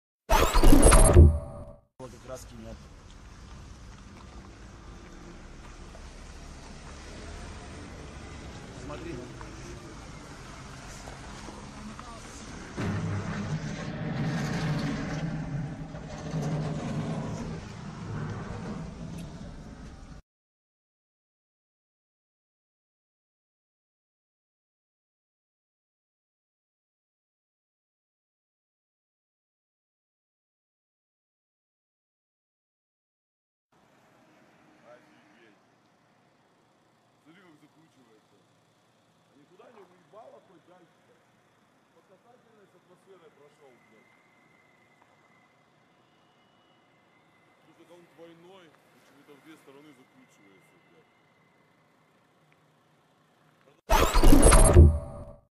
[Meteorito Cai na Russia] - Queda de meteorito na Rússia é a mais destrutiva dos últimos anos
Meteorito na Rússia afeta mais de 500 pessoas O meteorito caído na região de Chelyabinsk nesta sexta-feira (15) pesava dezenas de toneladas, disse o pesquisador sênior do Observatório de Pulkovo em São Petersburgo, Serguei Smirnov. O número de vítimas causadas pela queda passa de 500, segundo o Ministério das Situações de Emergência da Rússia.   "Até o presente momento, na região (de Chelyabinsk) foram registrados 514 casos de pessoas afetadas, onze delas foram hospitalizadas, incluindo duas crianças. A maioria dos afetados têm contusões e cortes e, portanto, não precisam de hospitalização. Os afetados receberam os cuidados médicos necessários", diz-se num comunicado difundido pelo centro de imprensa. Passagem de meteorito pela Rússia deixa pelo menos 400 feridos PUBLICIDADE DAS AGÊNCIAS DE NOTÍCIAS Pelo menos 400 pessoas ficaram feridas nesta sexta-feira após a passagem de um meteoro na região dos montes Urais, no centro da Rússia. O fenômeno natural causou a queda de bolas de fogo em direção à Terra, provocando tremores e a queda de bolas de fogo.